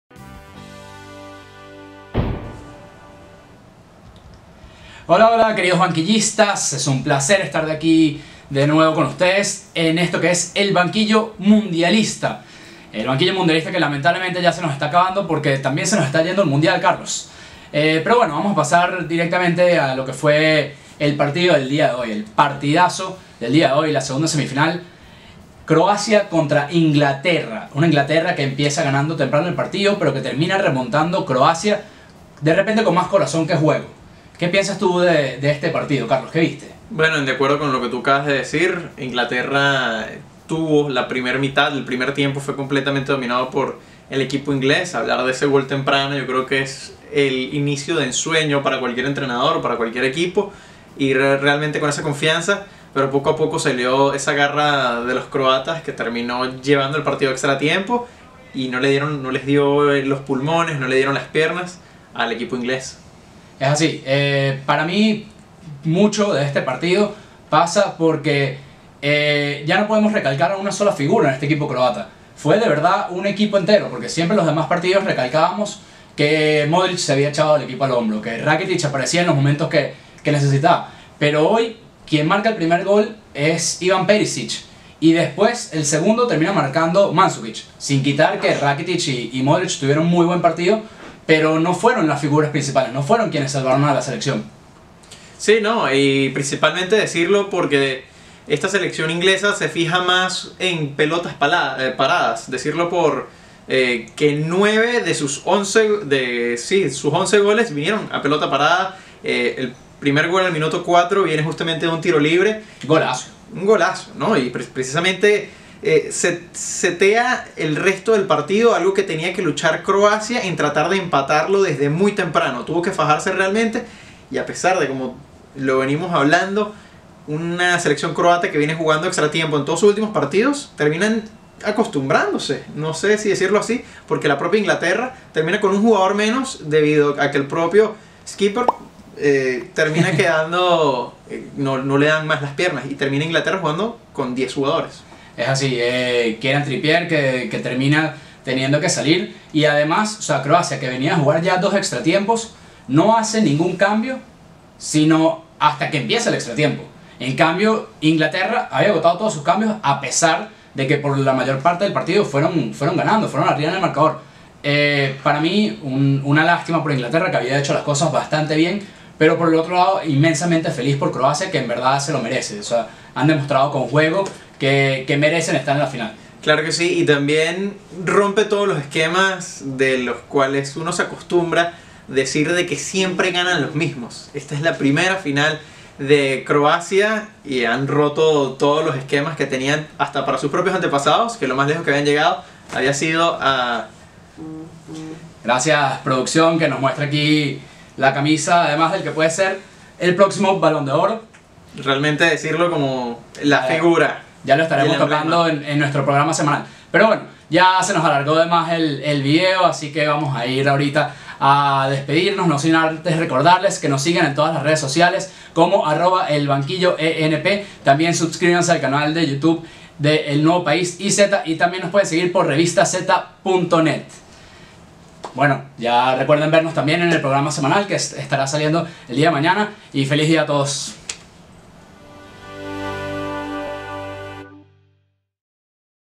¡Qué bonito es el fútbol! Croacia es FINALISTA (El Banquillo Mundialista 11-07-18)
Se acabaron las semifinales y se nos termina el mundial pero QUE MANERA DE TERMINAR.
Inglaterra y Croacia nos dieron un auténtico partidazo que se tuvo que definir en 120 minutos donde, al final, más pudo la garra y el despliegue croata que el empuje inglés por traer la copa a casa.
Carlos Pulido y Joaquin Brillembourg nos hablan de lo que fue este partido y cómo se vivió.